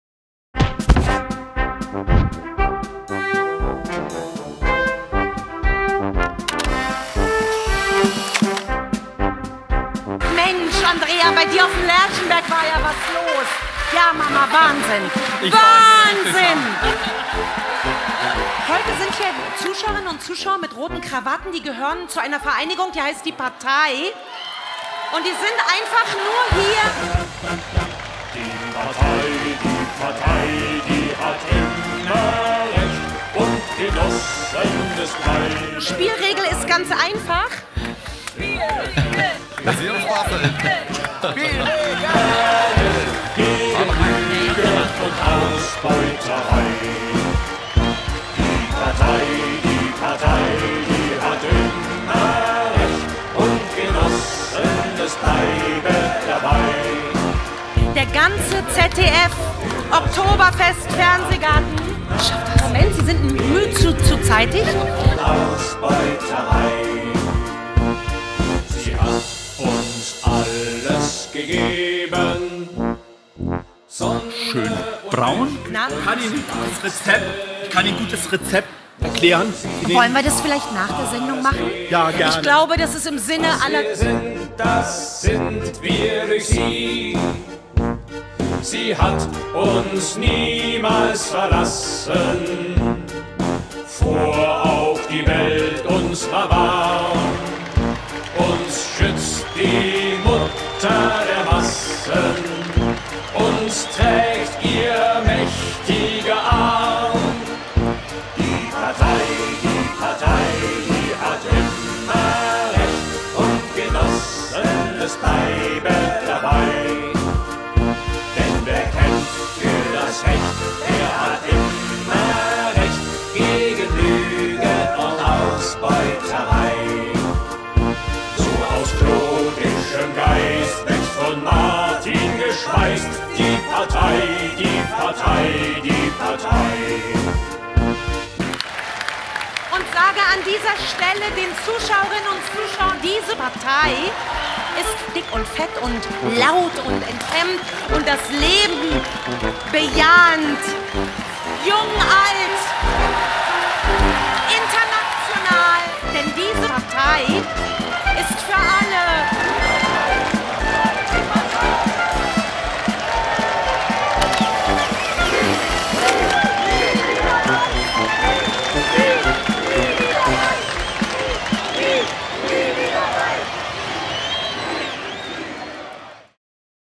Die PARTEI Poolparty
Den Abschluss des zermürbenden 10. Bundesparteitags der Partei Die PARTEI und der Badesaison 2023, feierte die Partei Die PARTEI im Freibad auf dem Mainzer Lerchenberg.
Das ZDF berichtete.

Sehr gute Informatiker*innen der PARTEI haben die TV- Übertragung durch  die PARTEI- KI gejagt.
Hier das Ergebnis.